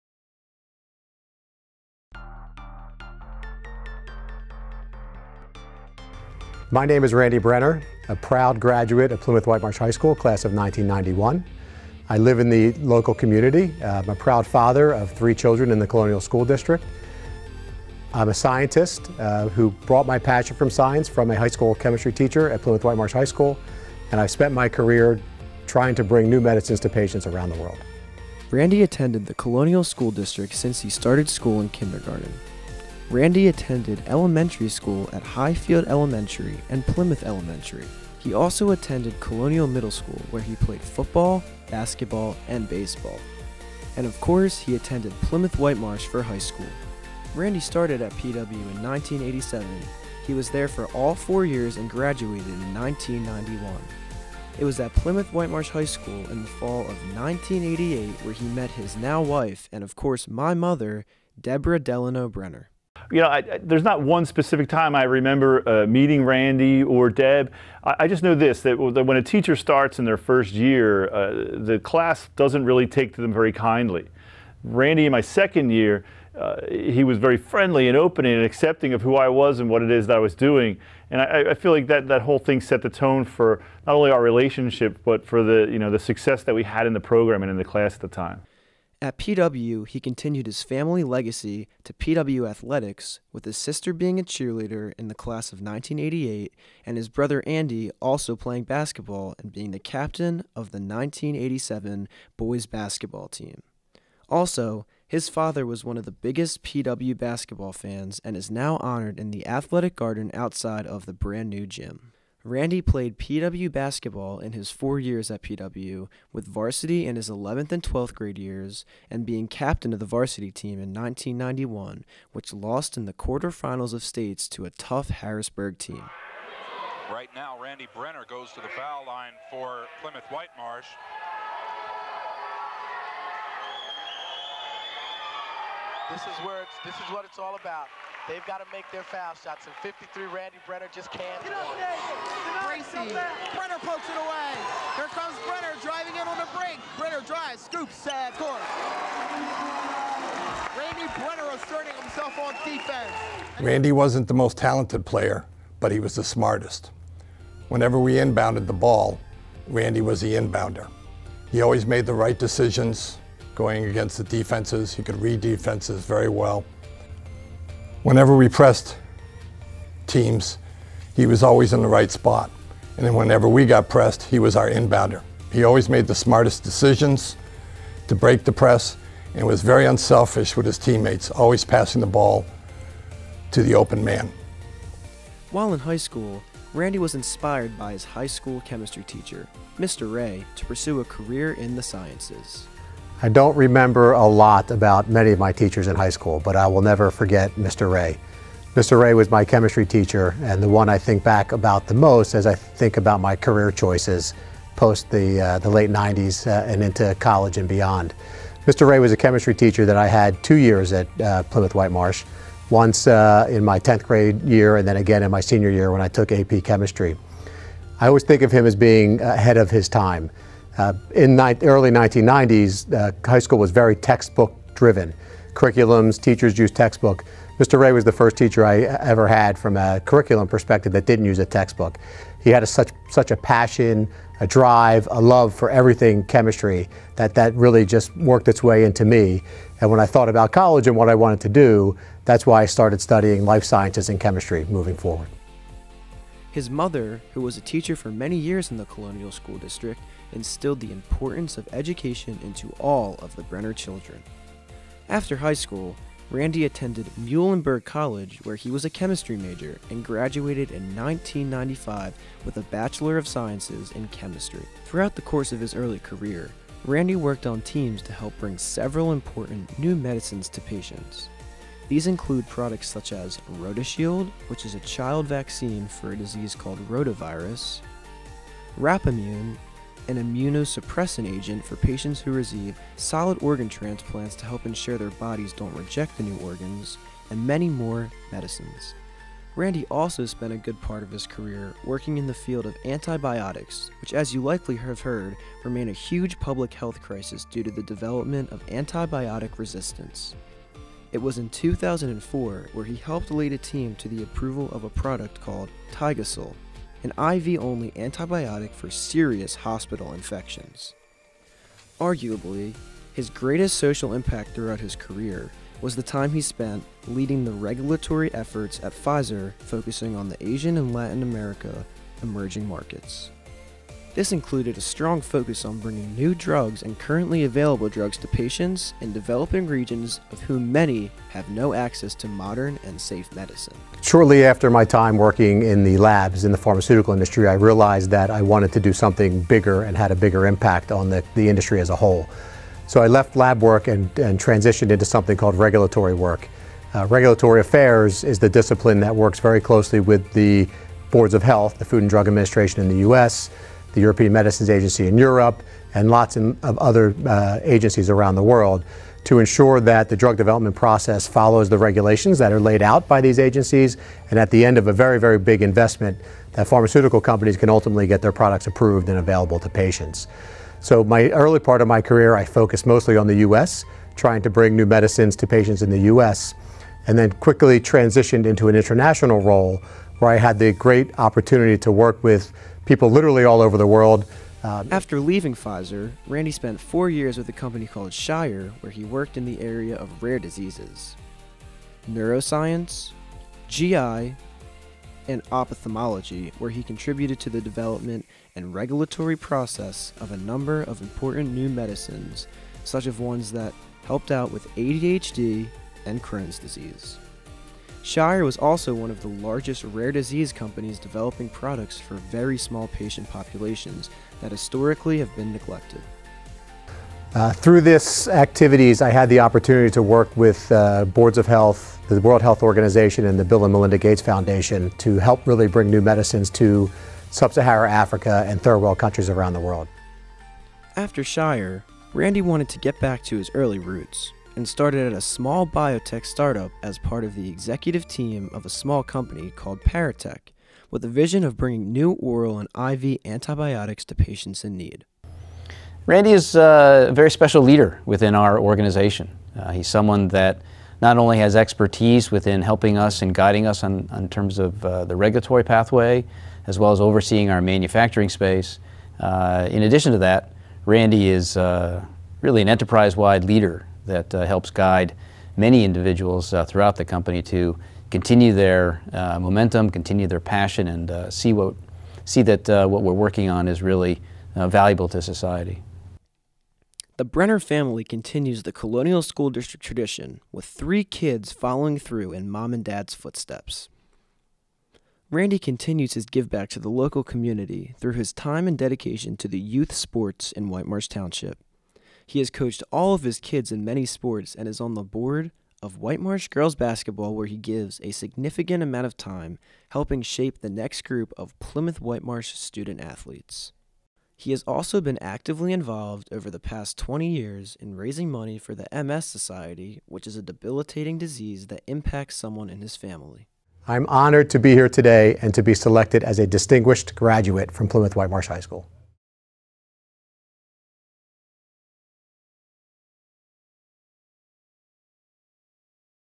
Randall Brenner (2019 Business)
Randy Brenner is a corporate leader in the pharmaceutical sector, specializing in Global Regulatory Affairs and Product Development.